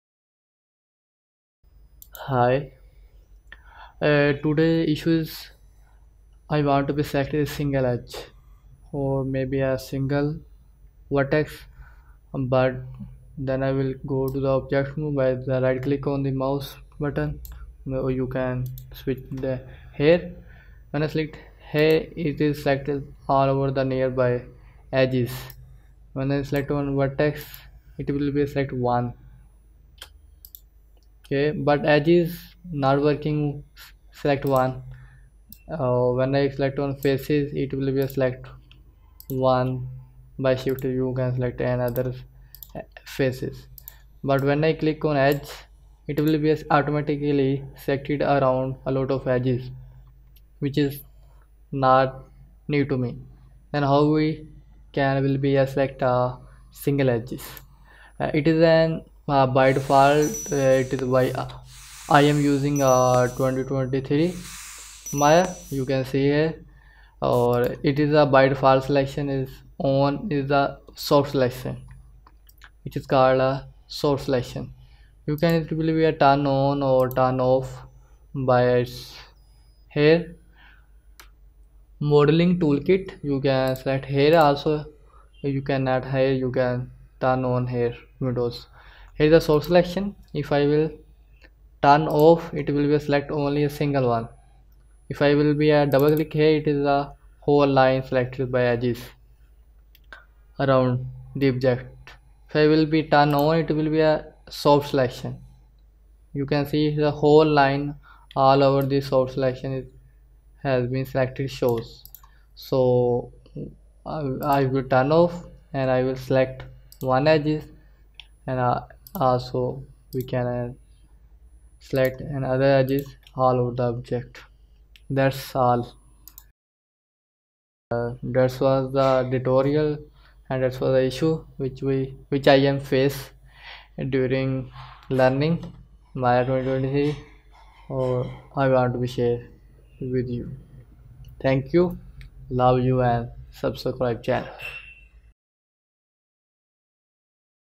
How to Select Single Edge Issue Solved In Maya Tutorial
How to Select Single Edge Issue, want to select, not all edges, Maya 2022, 2023, Solved

On request Also Make a Tutorial for you, My Team Tries Everything to Give you the latest information about IT Field and Related.'

 Thank Regards
"RN break"